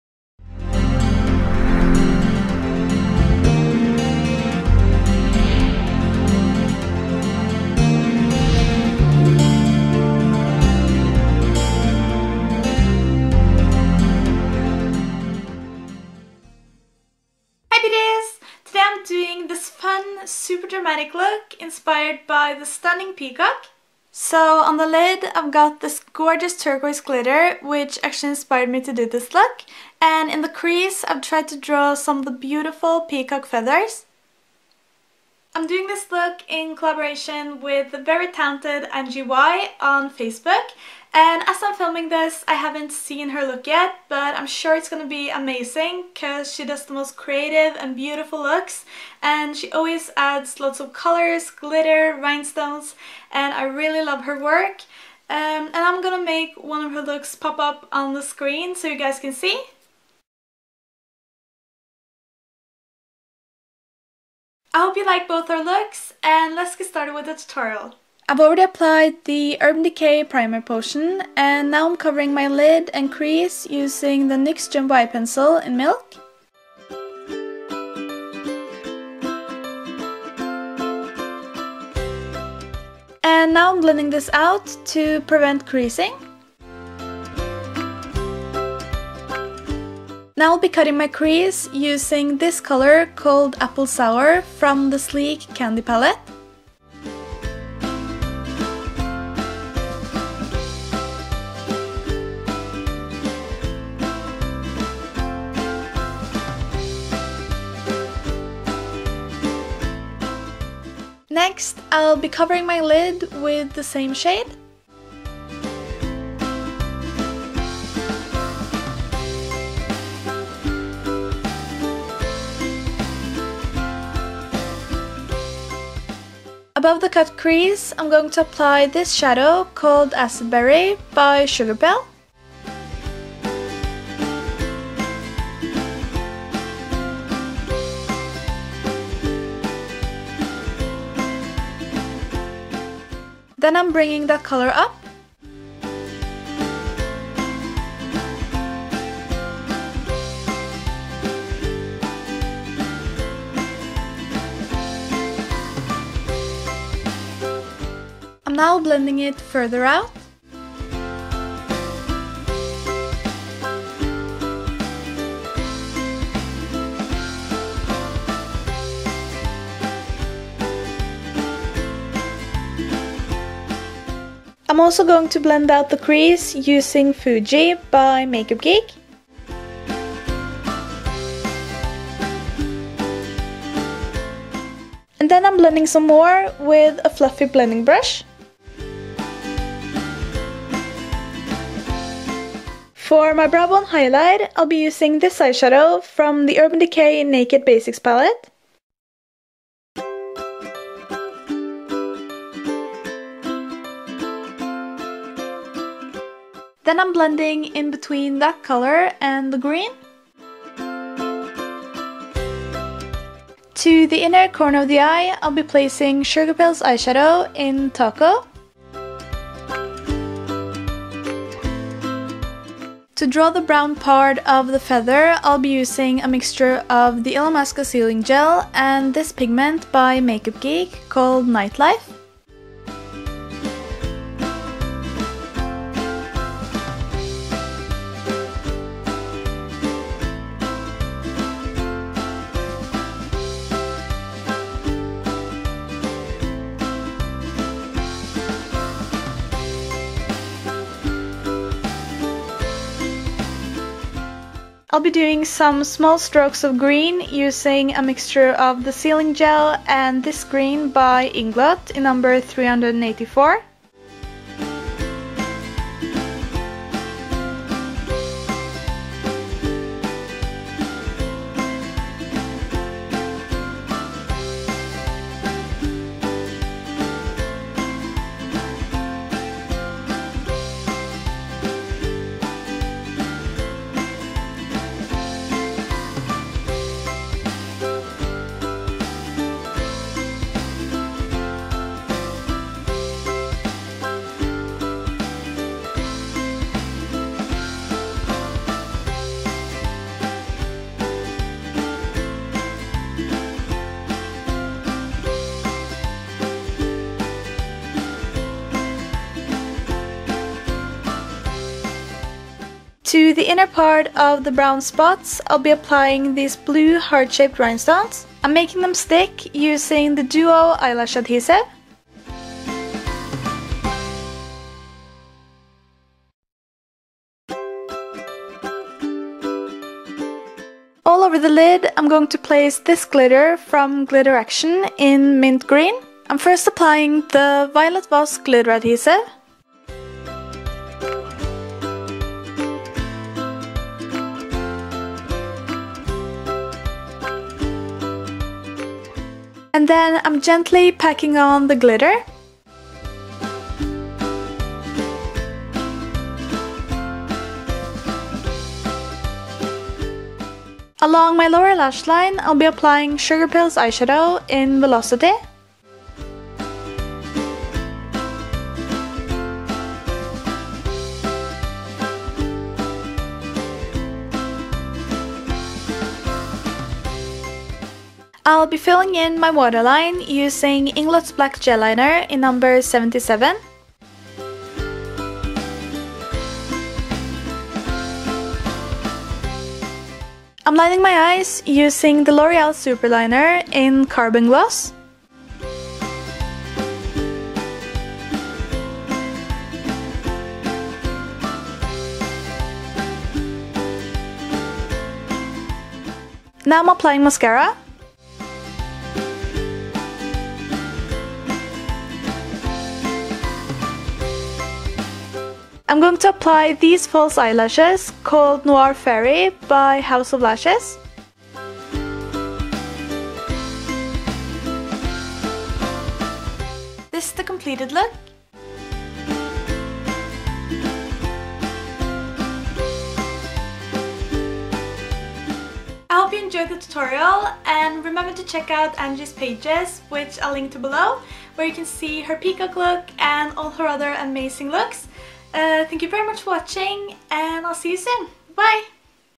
Peacock Makeup Tutorial
Products used:

DUO - Eyelash adhesive
Rhinestones from Ebay
L'Oreal - Super Liner in Carbon Gloss
Maybelline - Colossal Volum' Express mascara